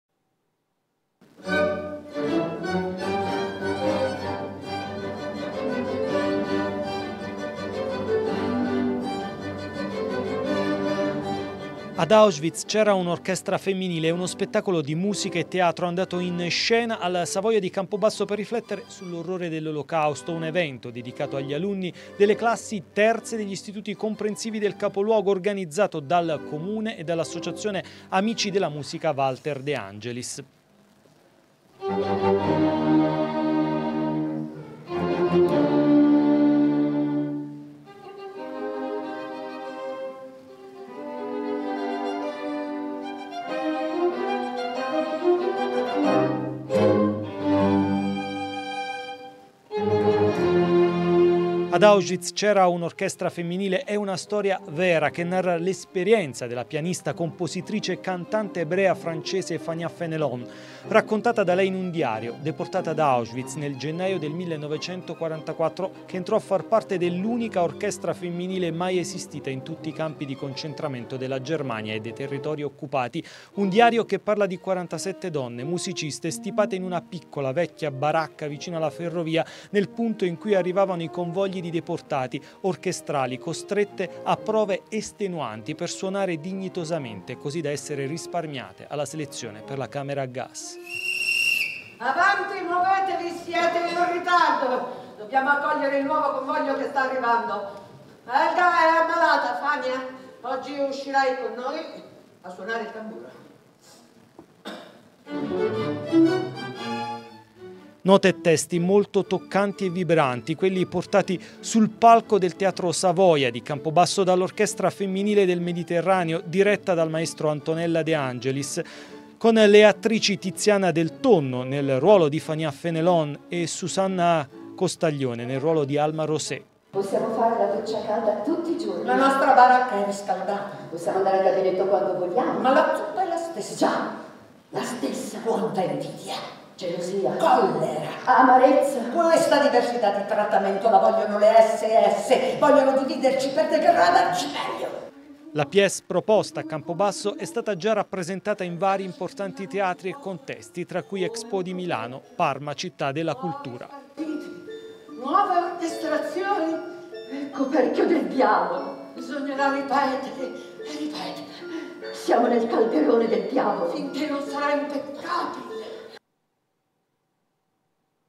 "Ad Auschwitz c'era un'orchestra femminile'', musica e teatro al Savoia
In occasione degli eventi correlati alla Giornata della Memoria, al Teatro Savoia, gli alunni delle classi terze degli Istituti Comprensivi di Campobasso, hanno assistito  gratuitamente allo spettacolo teatrale e musicale “Ad Auschwitz c’era un’orchestra femminile”, organizzato dal  Comune e dall'Associazione Amici della Musica Walter De Angelis.